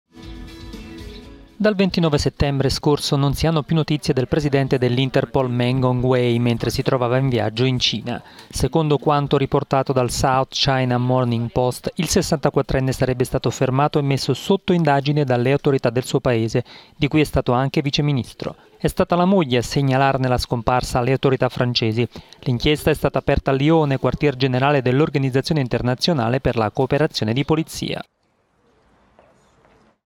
Cina: scomparso il Presidente dell'Interpol
Meng Hongwei, di cui la famiglia ha denunciato la mancanza di notizie dal 29 settembre mentre si trovava in viaggio in Cina, sarebbe stato messo sotto indagine dalle autorità del suo Paese…